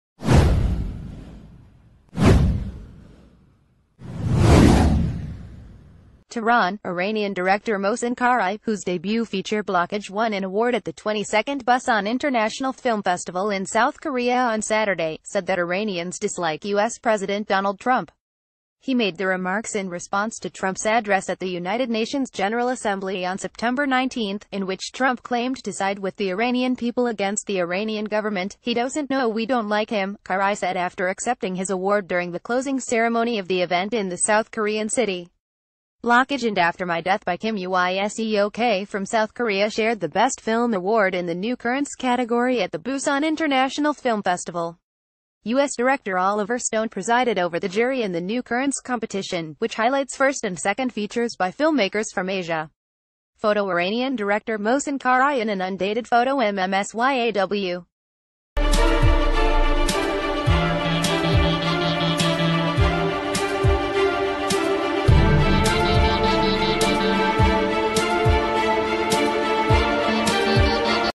Breaking News | Busan festival winner says iranians dislike trump
Busan festival winner says iranians dislike trump

TEHRAN – Iranian director Mohsen Qarai, whose debut feature “Blockage” won an award at the 22nd Busa...